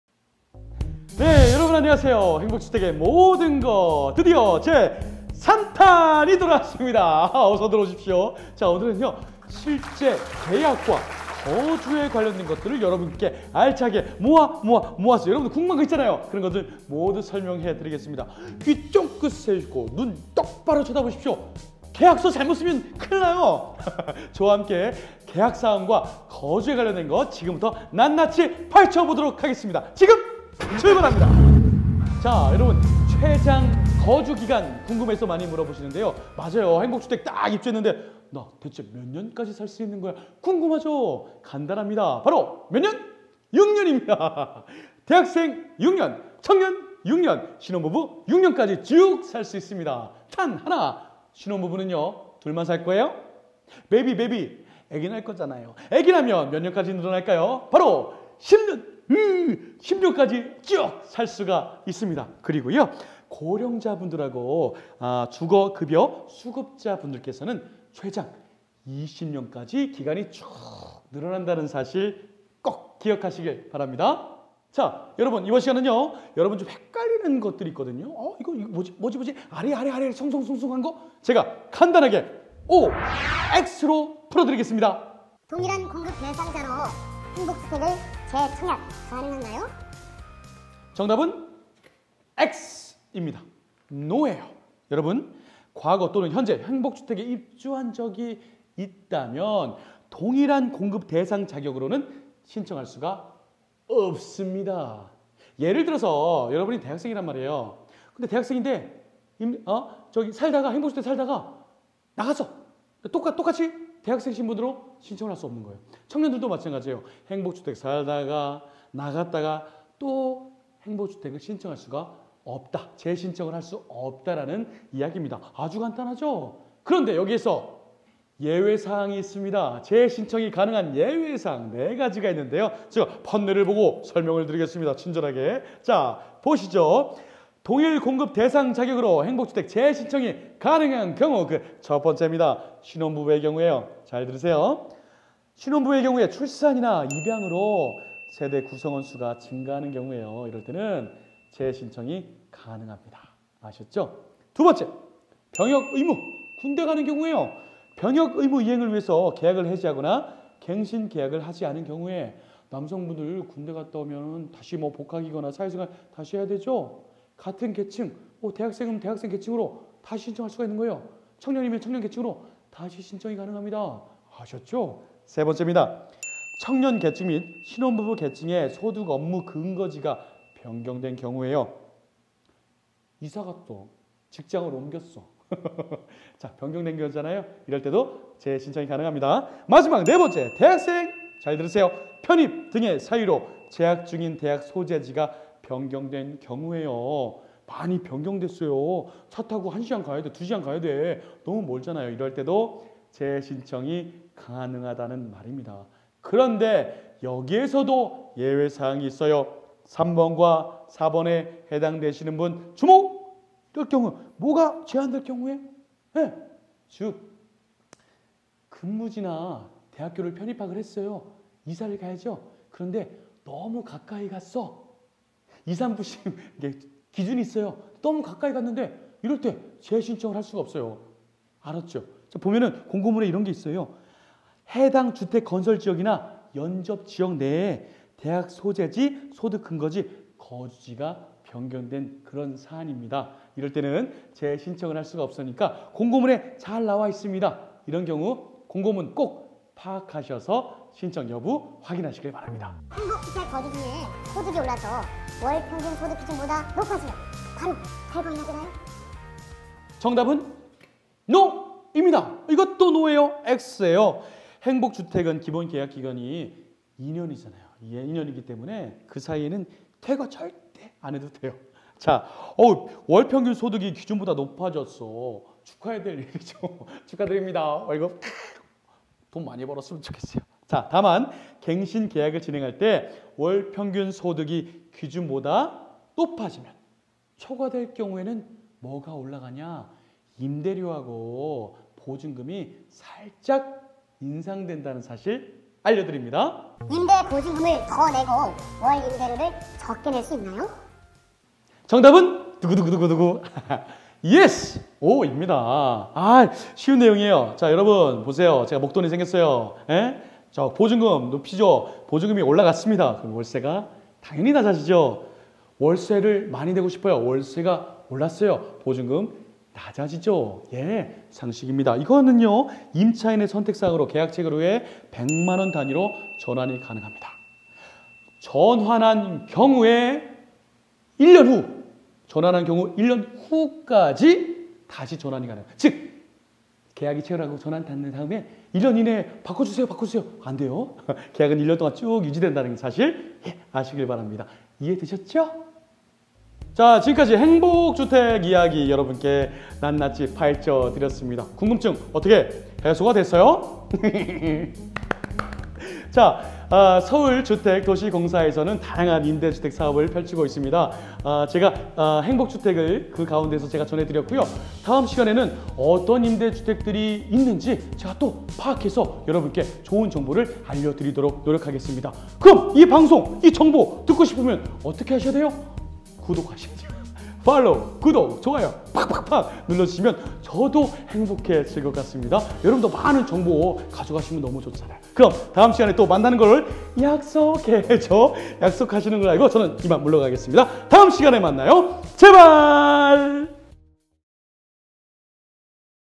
임대주택 궁금증을 모아모아! 행복주택의 모든 것 3편
대학생, 청년, 신혼부부를 위한 행복주택의 모든 것! 제 3편

"행복주택 영상 마지막! 궁금증을 시원하게 풀어보세요~!"
- 동일한 공급대상자격으로 행복주택을 재청약할 수 있나요?
- 월평균소득기준보다 소득이 높아지면 퇴거해야 하나요?
- 임대보증금을 더 내고 월 임대료를 적게 낼 수 있나요?

행복주택에 대한 궁금증을 모아 속 시원하게 답변해드립니다! :-)